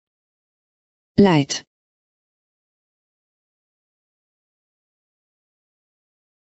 leidet
This is the Microsoft Hedda voice that is reading the word leidet